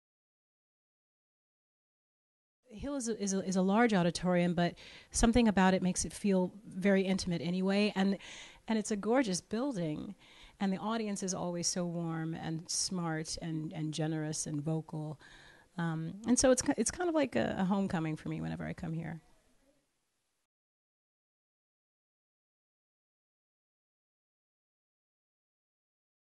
UMS Presents: Audra McDonald on Hill Auditorium
Singer Audra McDonald shares her feelings about Hill Auditorium and Ann Arbor audiences.